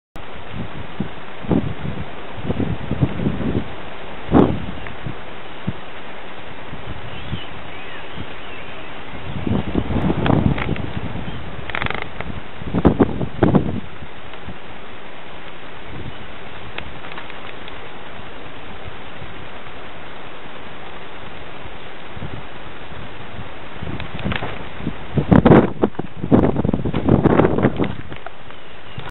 monorail cat foiled by snow
fluffy foild by snow and has to take a detour